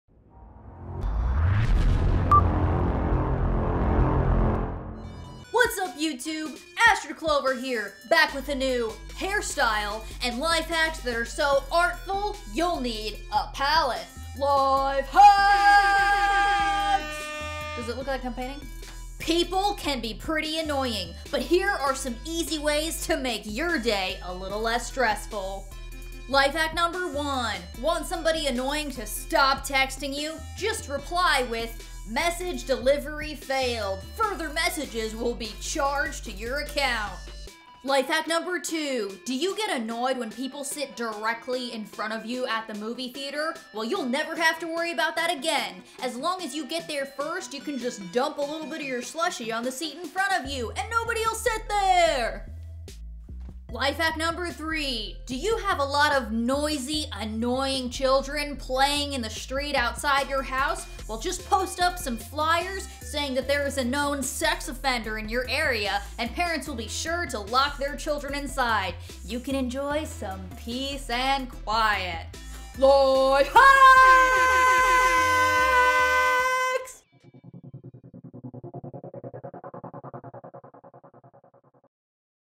Life Hacks with Astrid - Annoying People
Astrid shares some life hacks to deal with annoying people.